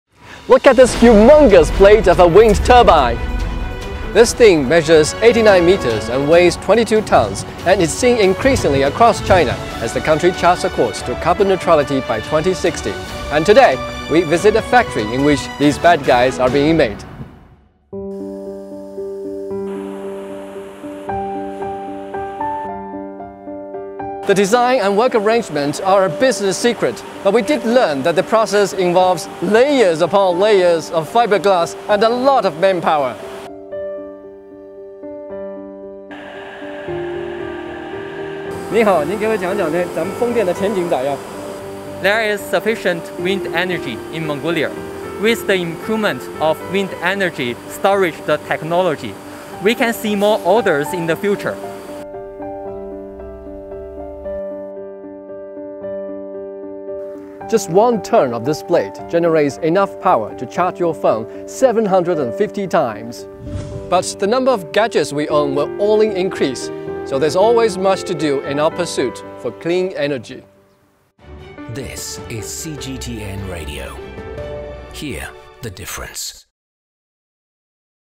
The wonderful wind turbines
Ever wondered how much electricity these wind turbines can generate? Will they be a reliable source of energy as we step up efforts to reach carbon neutrality and combat climate change? Watch this to get informed on technologies that will change your lives.